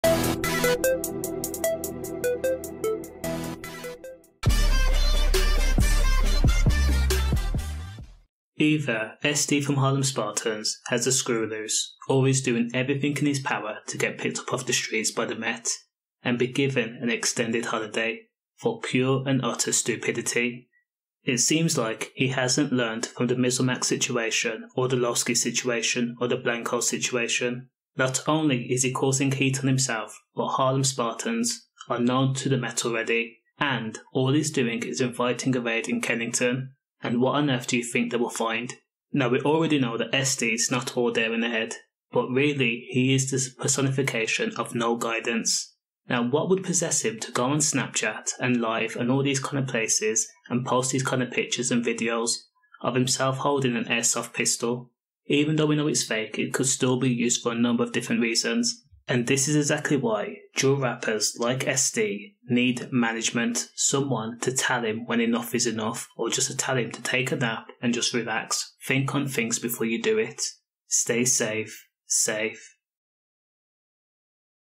SD Harlem Spartans doing everything he can to invite the MET to Kennington
SDfrom Harlem spartans has not learned from mizormac's, loski, blanco's mistake and is doing everything he can to get on the METs radar